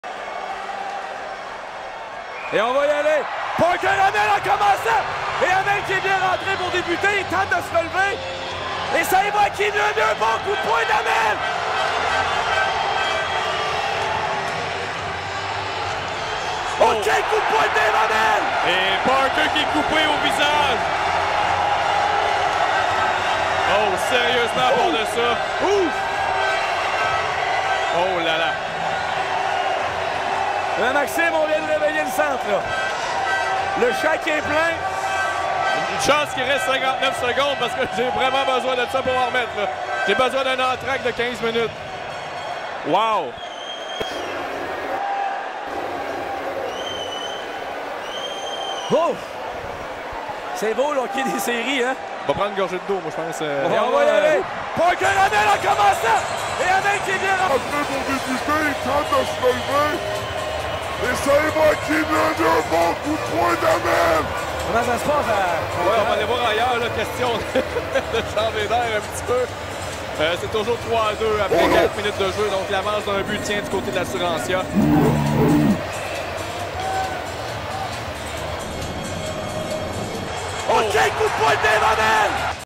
Dave Hamel vs Derek Parker (Round 7 of Playoffs!) - 15-04-23
Saint-Georges vs Laval - Demi-Finale, Match #4